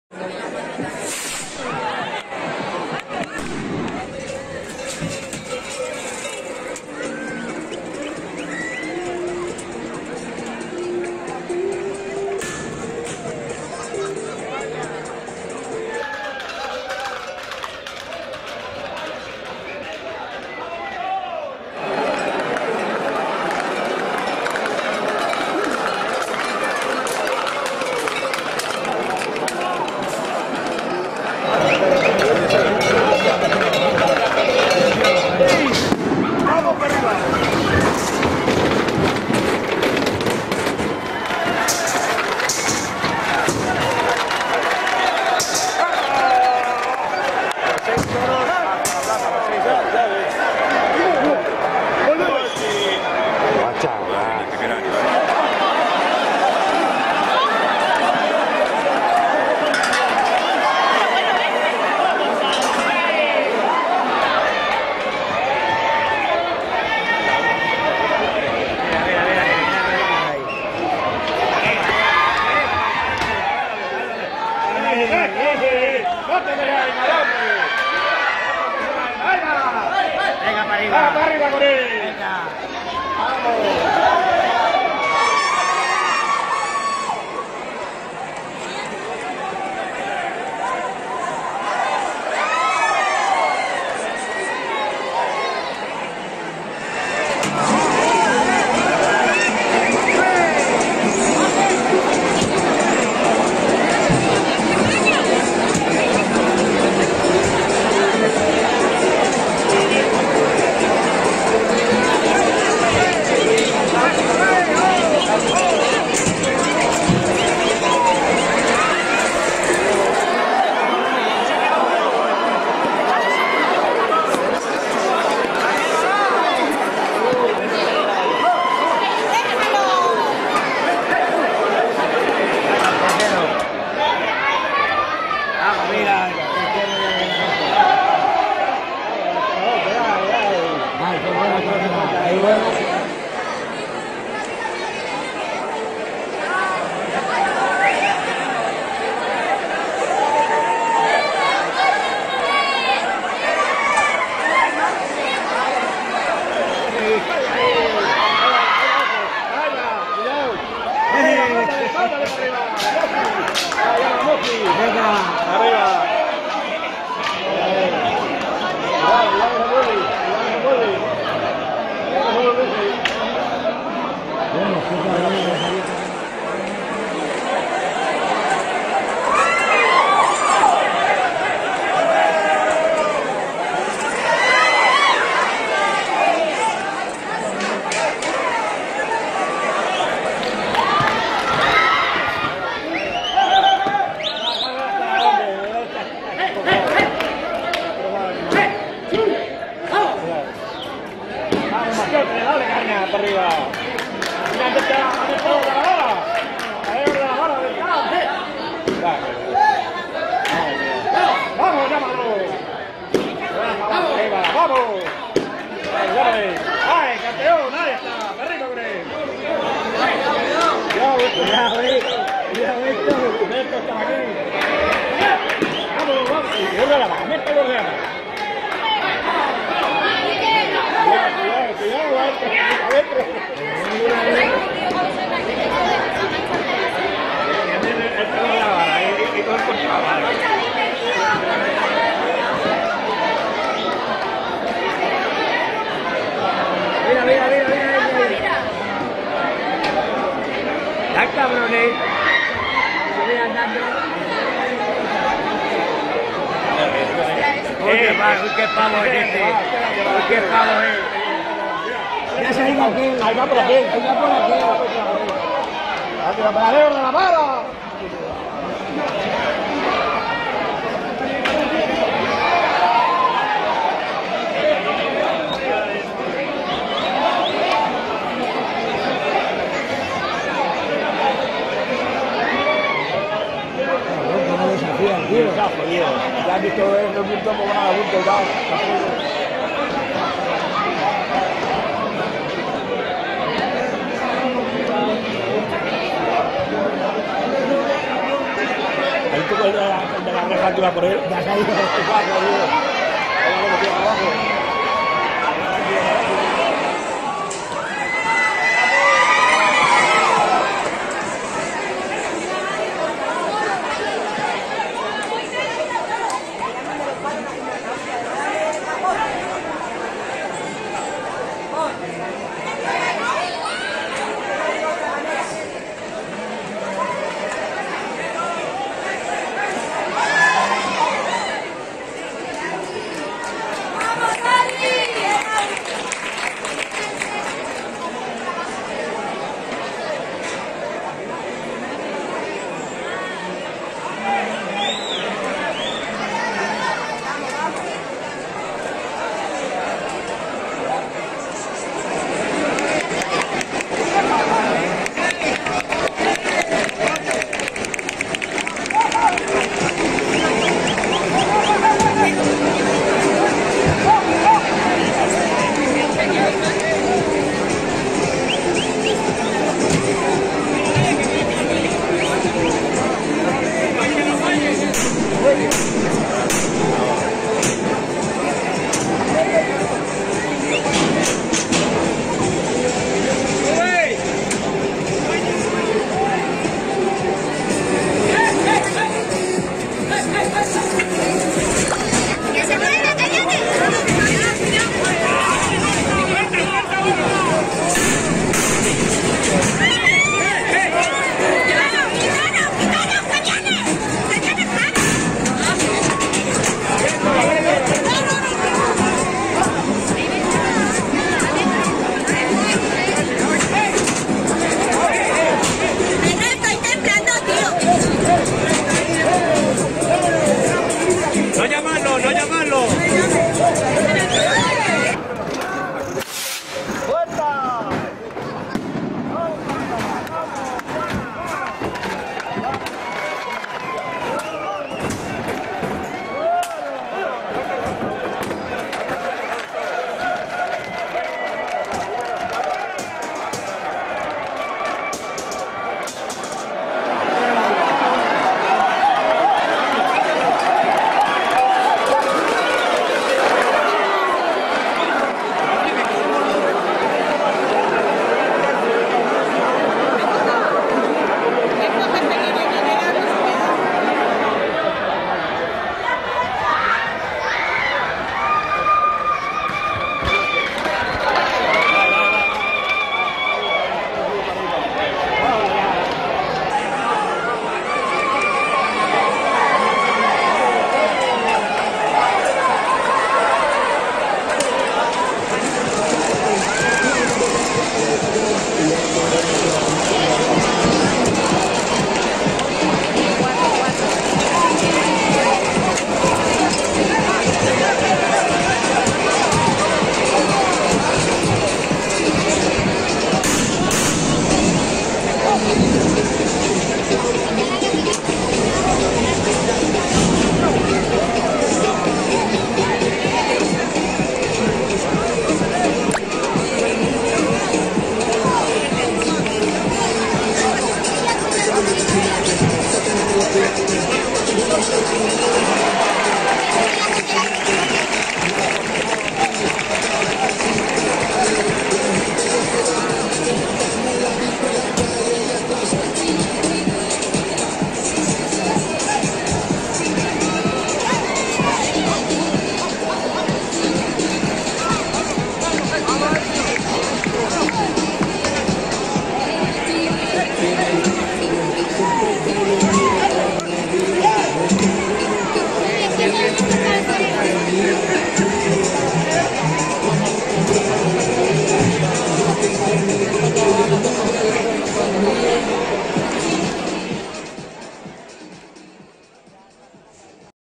Primer encierro de las Fiestas de Arganda 2022. Ana Romero.
Primer encierro de las FiestasArganda2022 con novillos de Ana Romero sin ningún incidente reseñable